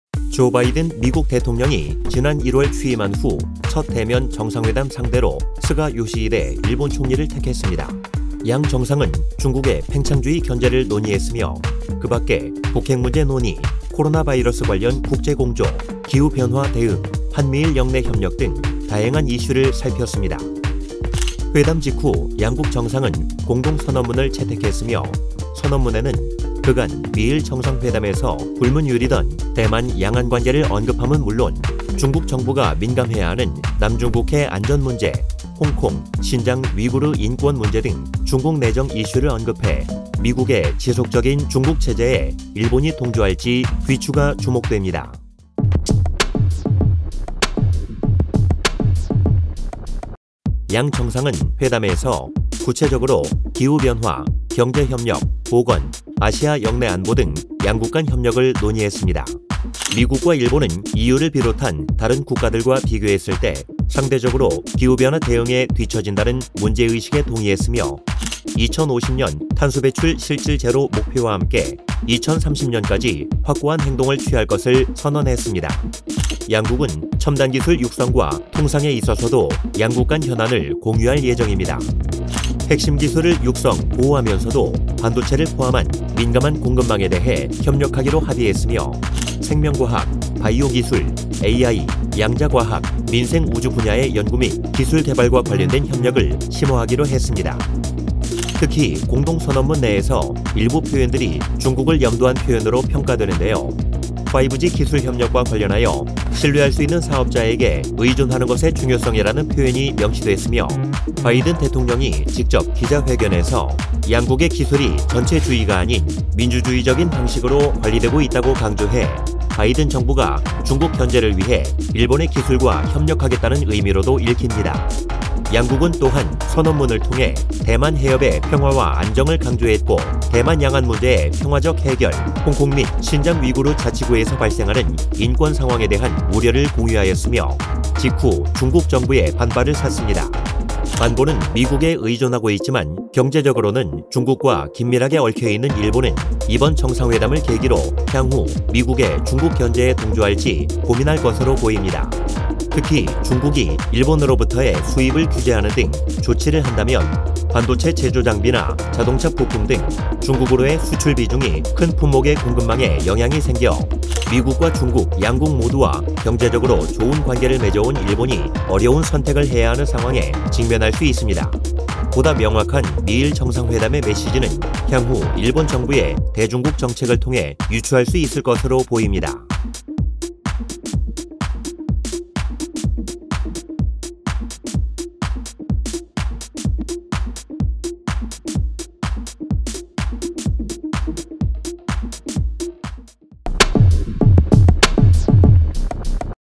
Vol.53 미국과 중국 사이, 고심하는 일본 (+미일정상회담, 일본의 대중국정책은?)
조 바이든 미국 대통령이, 지난 1월 취임한 후
첫 대면 정상회담 상대로
스가 요시히데 일본 총리를 택했습니다.

양 정상은 중국의 팽창주의 견제를 논의했으며,
그밖에 북핵문제 논의, 코로나바이러스 관련 국제공조,
기후변화 대응, 한·미·일 역내 협력 등 다양한 이슈를 살폈습니다.

회담 직후, 양국 정상은 공동선언문을 채택했으며,
선언문에는 그간 미일정상회담에서 불문율이던
대만 양안관계를 언급함은 물론,
중국정부가 민감해야하는 남중국해 안전문제,
홍콩, 신장 위구르 인권문제 등 중국내정 이슈를 언급해,
미국의 지속적인 중국 제재에 일본이 동조할지 귀추가 주목됩니다.